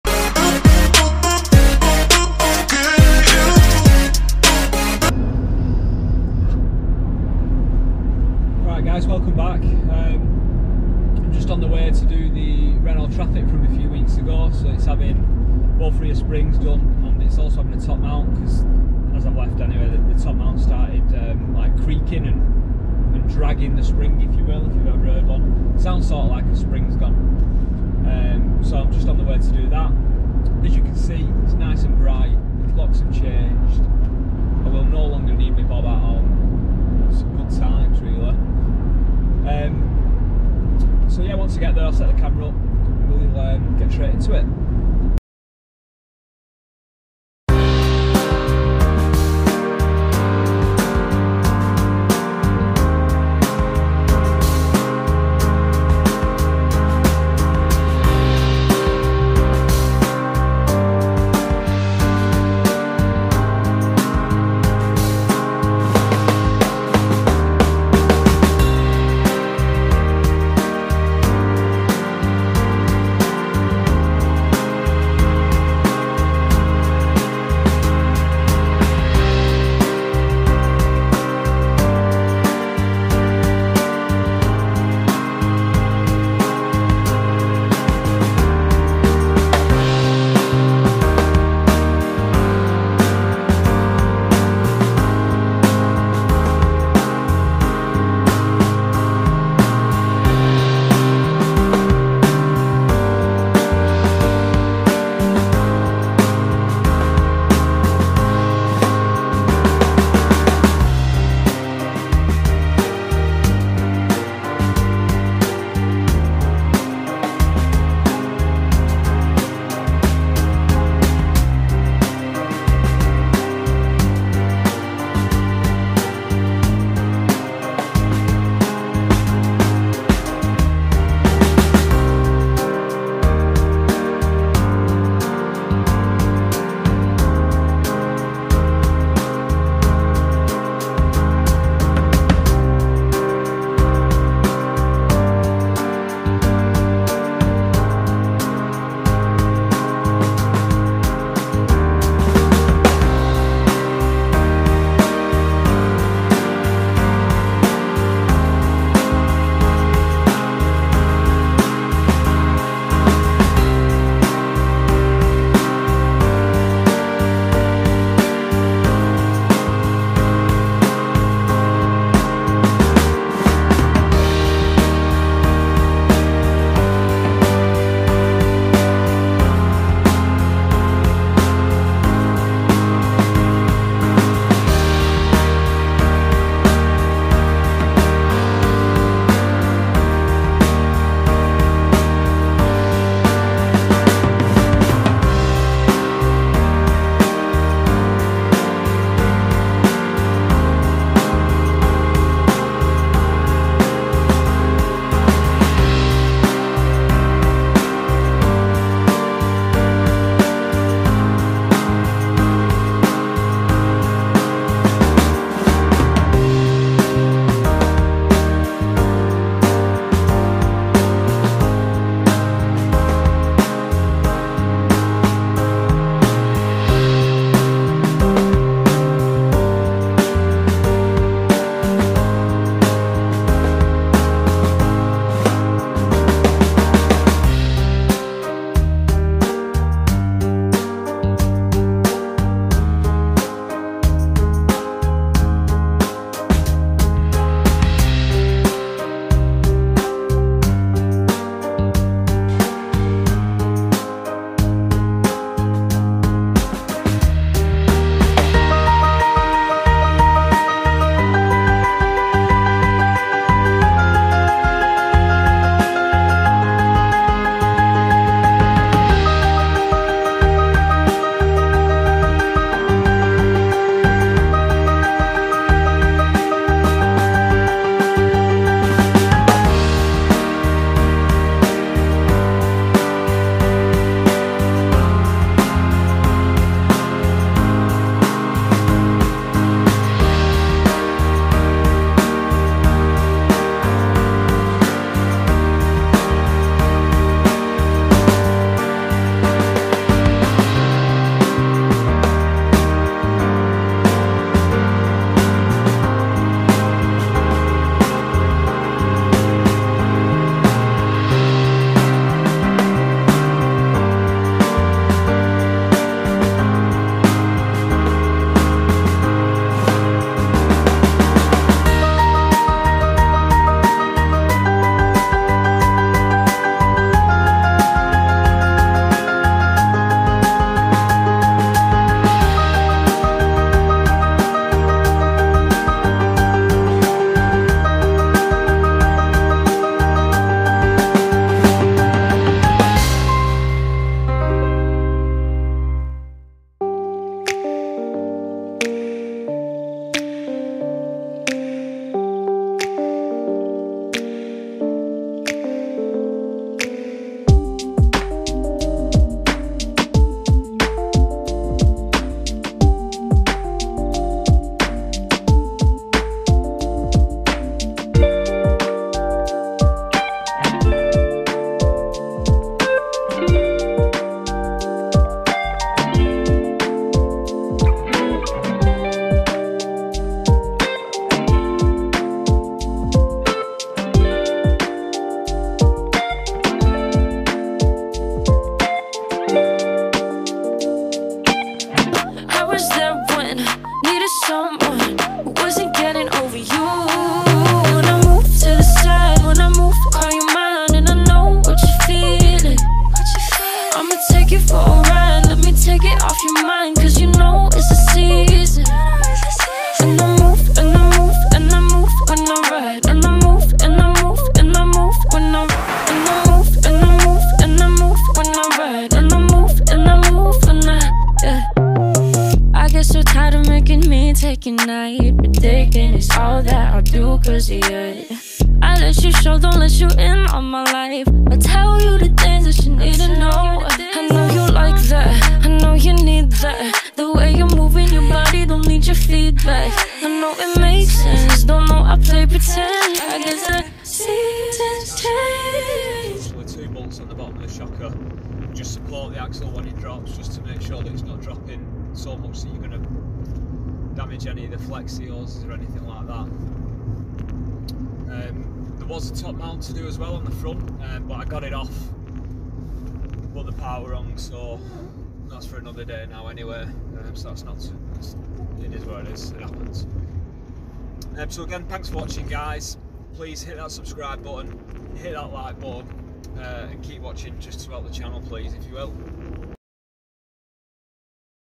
Rear coil spring replacement. Real Time 6 mins
Such a easy job to replace the rear springs on a reanult traffic. Once the van was in the air it took 6 mins for both rear springs.